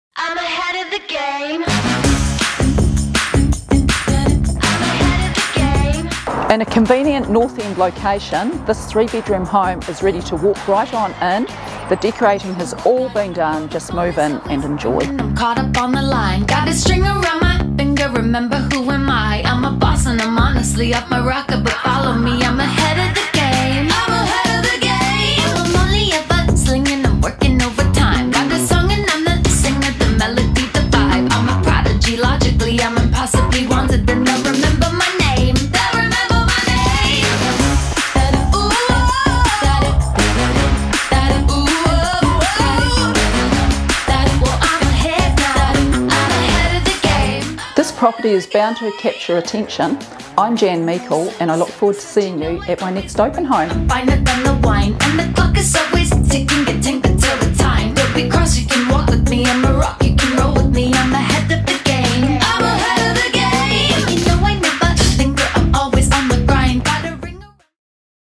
489 Thames Highway, Oamaru | Price by Negotiation over $395,000
Walk Right On In

The decorating has all been done, just move on in and enjoy easy living in this very tidy, beautifully presented home.

The living area is bathed in afternoon sun, adjoins the modern kitchen, and has heat pump and log-burner for the cooler evenings.

There are three bedrooms all with built in wardrobes, family bathroom and separate laundry.

Outdoor living has options, with a small sheltered area to the front of the home and a barbeque area out the back.

The section is flat, fenced, has a garden shed, woodshed and a freestanding double garage with power and work-bench.

Plenty of off-street parking, allows room to park up the caravan or boat, or play space for the children or pets.

First home buyers, down-sizers, or investors this is one to view.

blackhamandco.co.nz/oam421